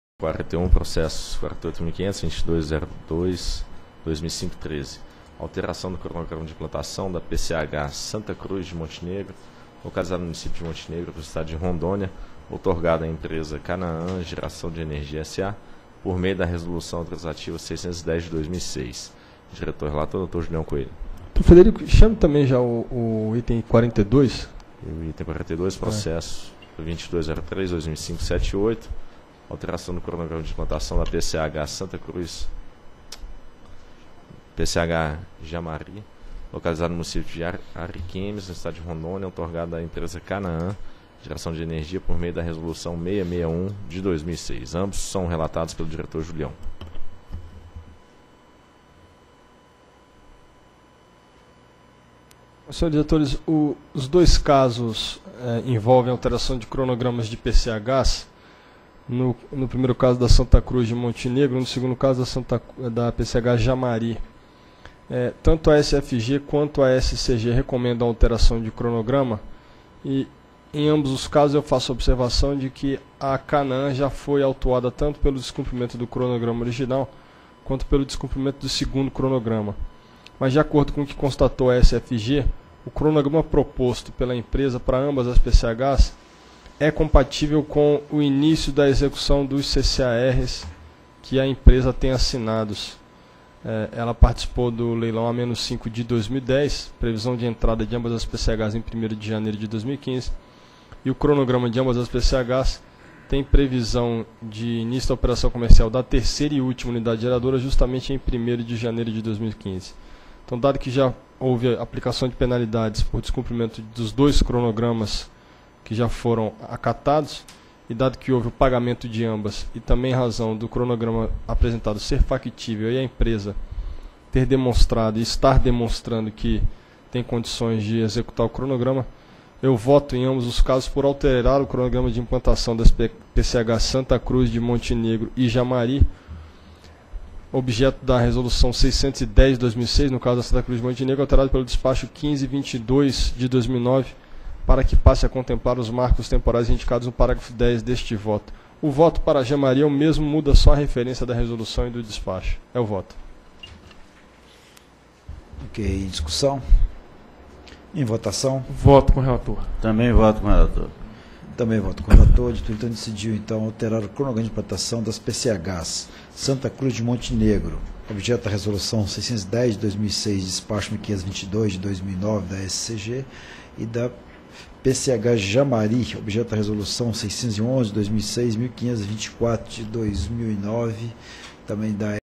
Item 41 e 42 - 23ª Reunião Pública Ordinária ANEEL 26/06/2012
Item 41. Processo nº: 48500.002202/2005-13. Assunto: Alteração do cronograma de implantação da Pequena Central Hidrelétrica -- PCH Santa Cruz de Montenegro, localizada no município de Monte Negro, no estado de Rondônia, outorgada à empresa Canaã Geração de Energia S.A. por meio da Resolução Autorizativa nº 610/2006. Área Responsável: Superintendência de Concessões e Autorizações de Geração -- SCG.

Item 42. Processo nº: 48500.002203/2005-78. Assunto: Alteração do cronograma de implantação da Pequena Central Hidrelétrica -- PCH Jamari, localizada no município de Ariquemes, no estado de Rondônia, outorgada à empresa Canaã Geração de Energia S.A. por meio da Resolução Autorizativa nº 611/2006. Área Responsável: Superintendência de Concessões e Autorizações de Geração -- SCG.